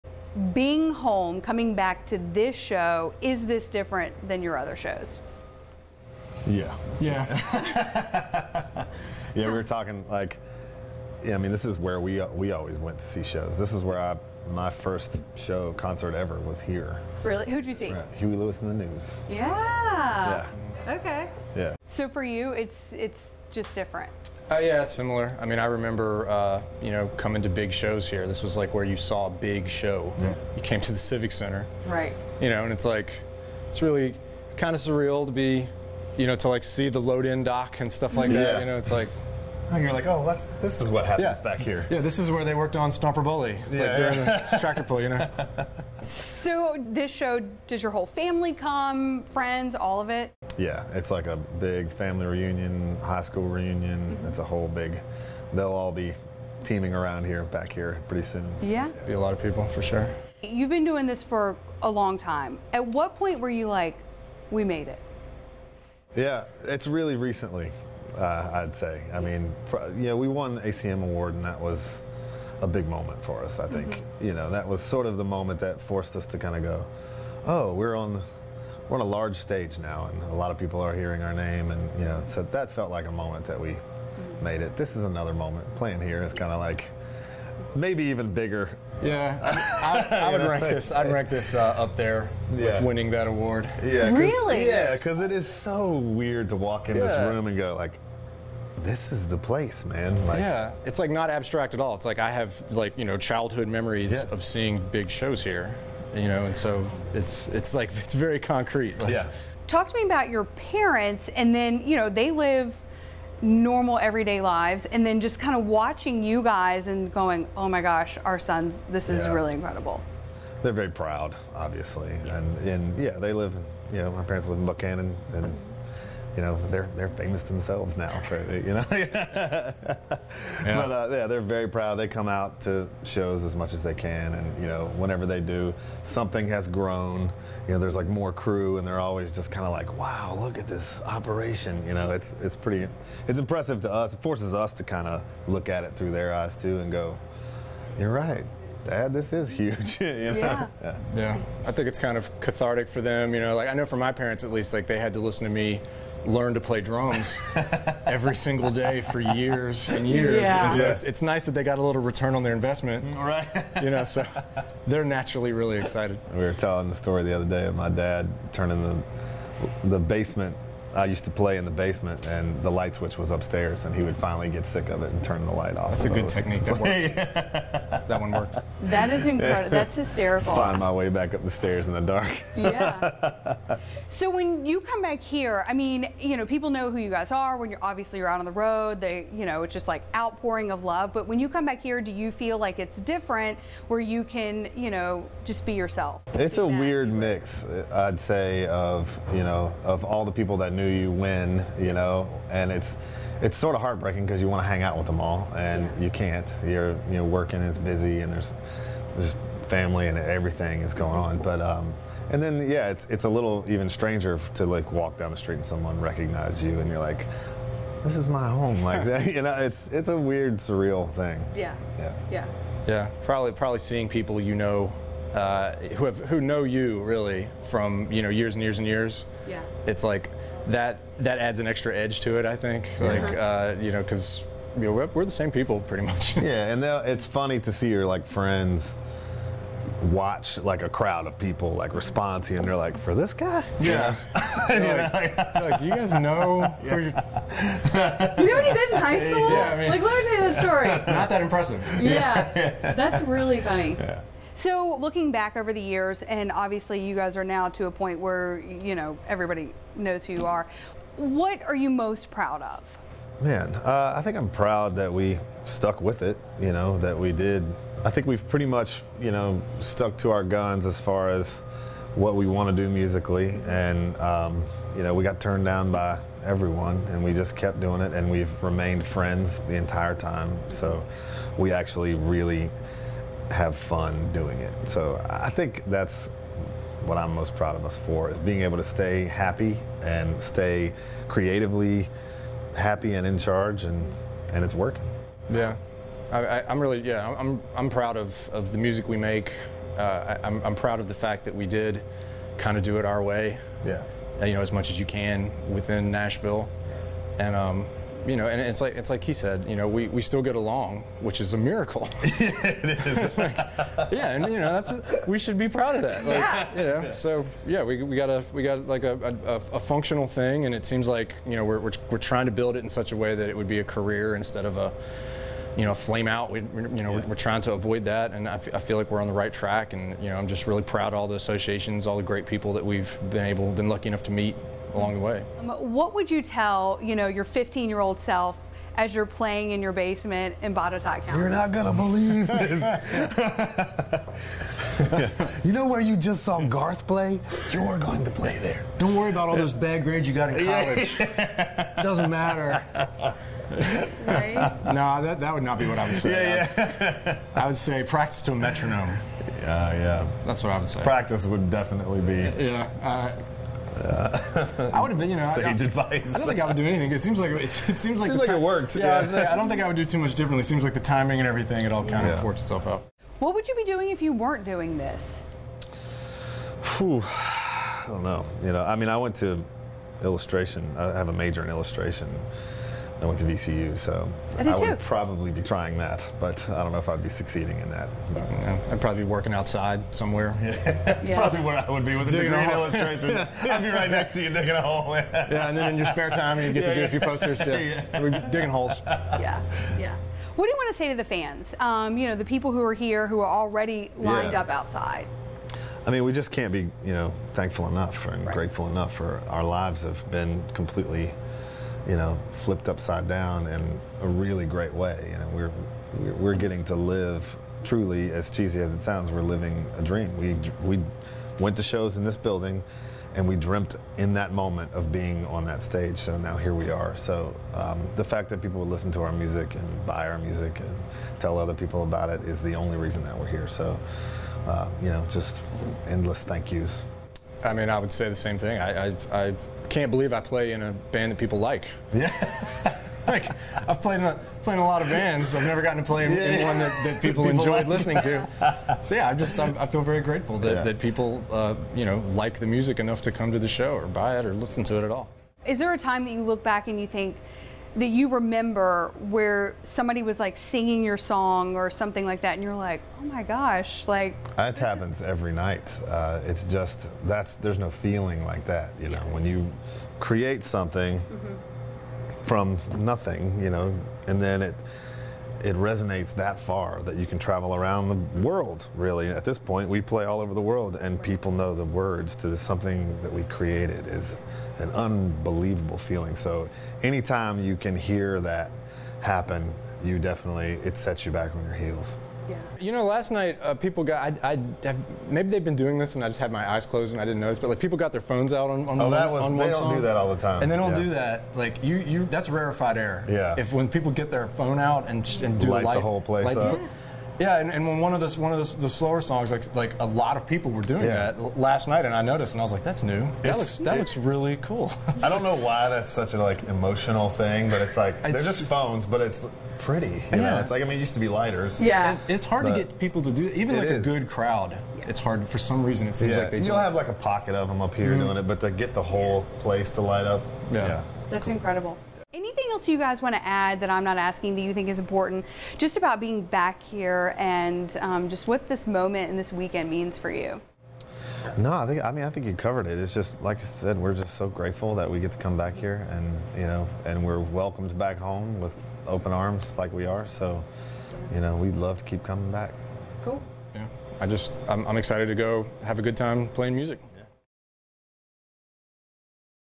More from Lindsey Ward's interview with Old Dominion's Matthew Ramsey and Whit Sellers
Lindsey Ward was able to sit down with two members of the popular country music band.